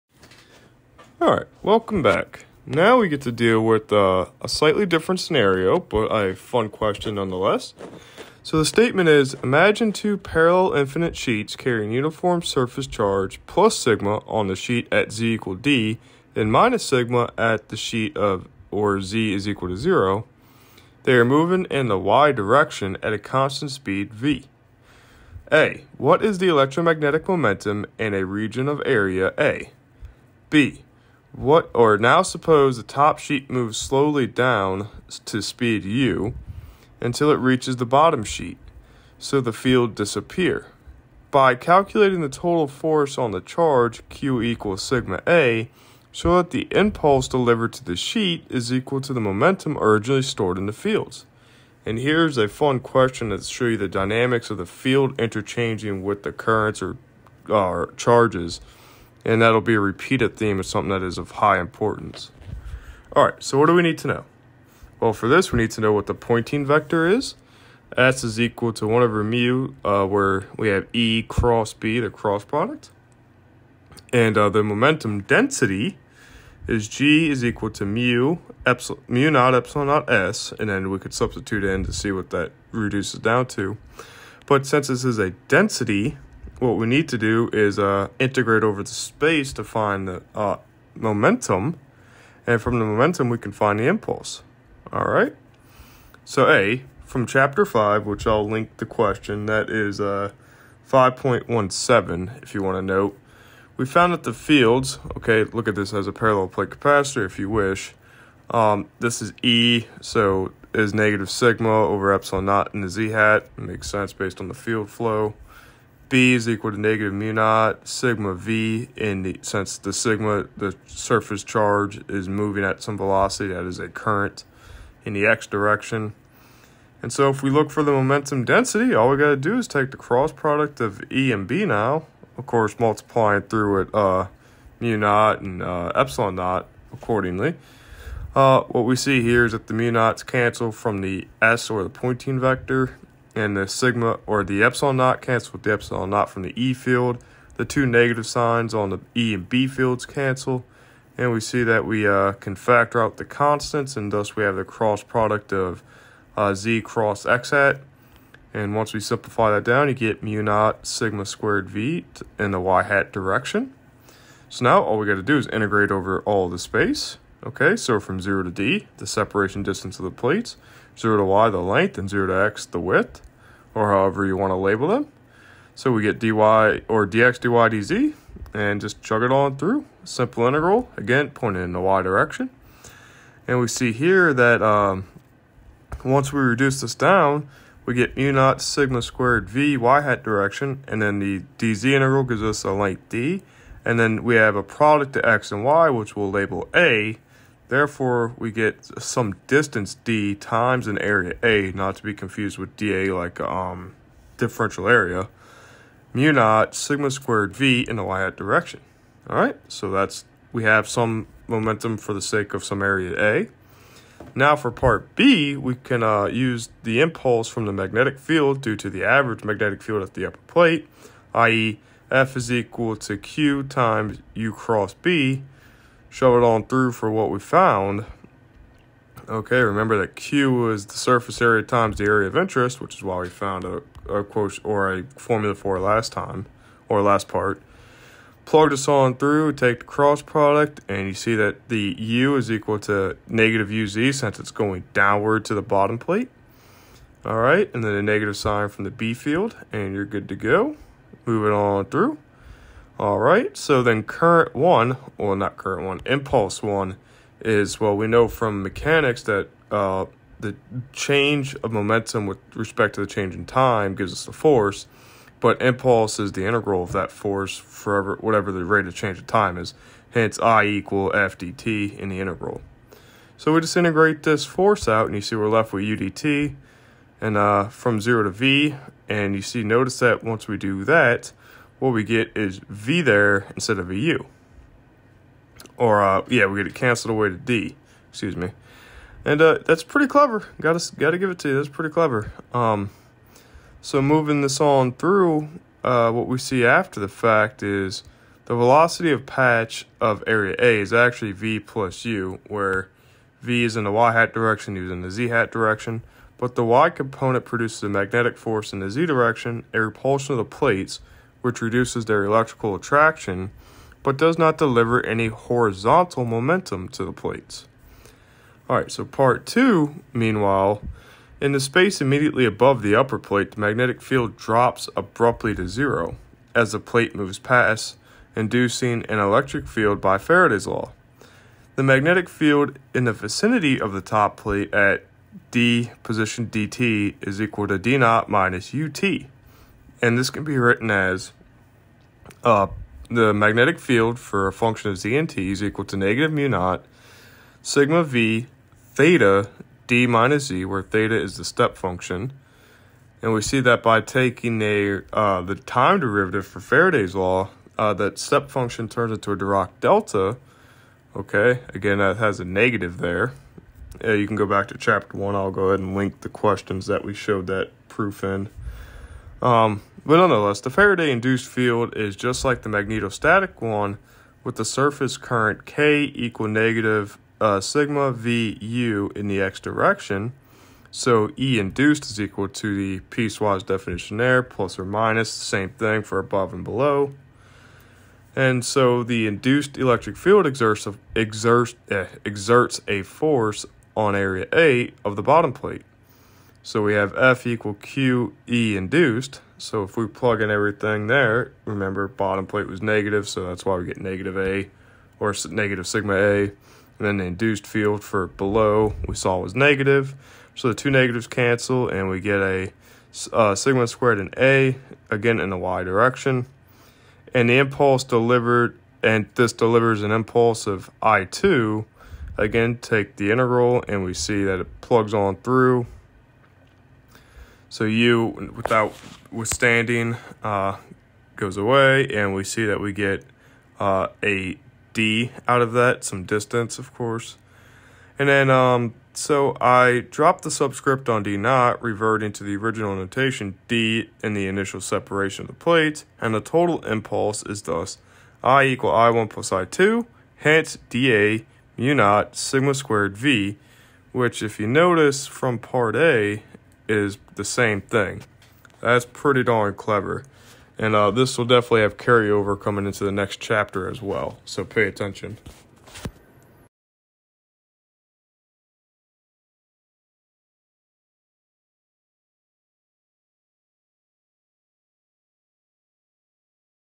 Problem 8.5 - Momentum, Conservation of Momentum: Introduction to Electrodynamics
As the upper plate passes by, the magnetic field drops to zero, inducing an electric field that delivers an impulse to the lower plate. This setup makes use of another question we already compiled from chapter 5, (5.17

While part (a) is pretty quick to capture with the tools of chapter 8, part (b) plays on the things from chapter 7 as well and it is just so darn awesome to see it come together like this!  The impulse on the lower plate exactly matches the momentum initially stored in the fields! How cool is that?! Further it is worth noting that:

"Incidentally, there is also 'hidden momentum' in the original configuration. It is not relevant here; it is (relativistic) mechanical momentum and is delivered to the plates as they come together, so it does not affect the overall conservation of momentum."

Cannot wait to get into chapter 12 with you all!

☞ 📚📖📓= Griffiths, David J. “Chapter 8 Conservation Laws.” 𝘐𝘯𝘵𝘳𝘰𝘥𝘶𝘤𝘵𝘪𝘰𝘯 𝘵𝘰 𝘌𝘭𝘦𝘤𝘵𝘳𝘰𝘥𝘺𝘯𝘢𝘮𝘪𝘤𝘴, Pearson, Boston, MA, 2014, pp. 356–381.-